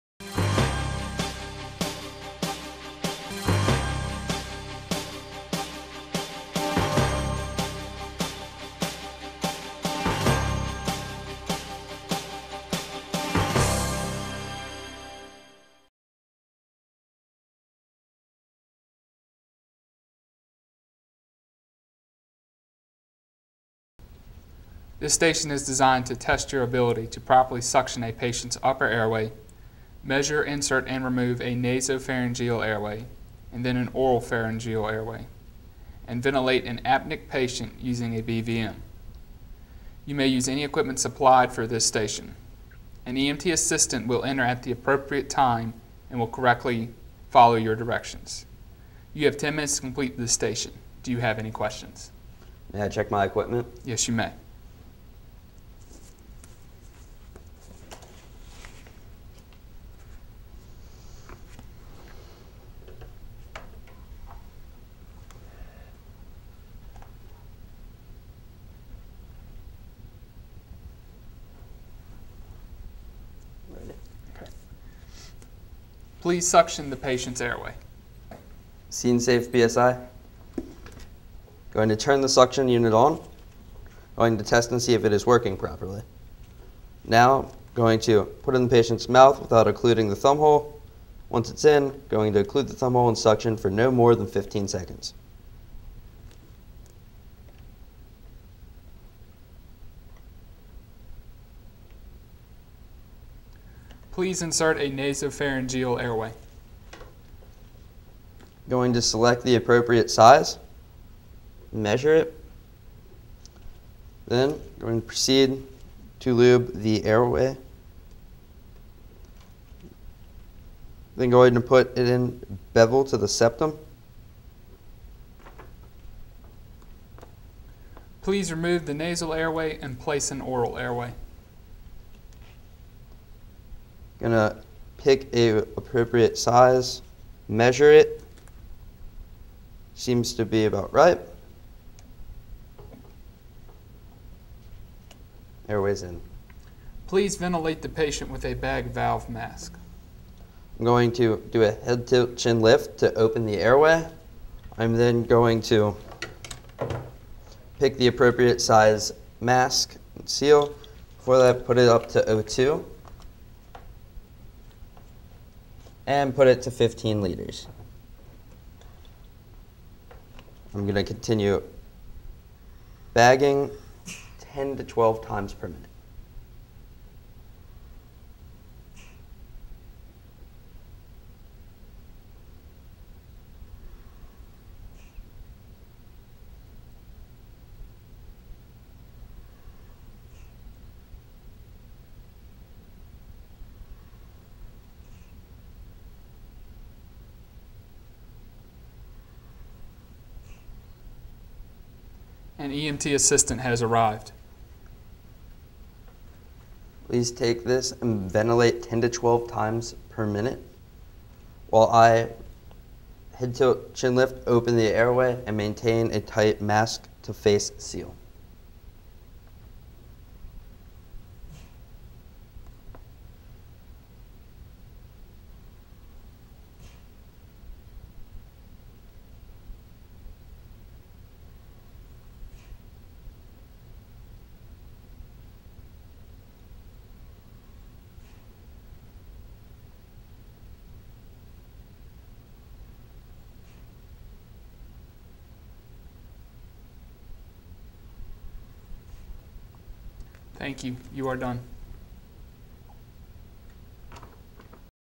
VAOEMS 3 of 9, Psychomotor Exam -- Airway, Oxygen & Ventilatory Support
The Virginia Office of EMS presents a nine-part video series on the EMS Psychomotor Testing Experience in the Commonwealth. This video provides an example of what the EMT candidate may experience during the Airway, Oxygen & Ventilatory Support, Psychomotor Station Testing.  It is not intended to be used as a skill teaching tool.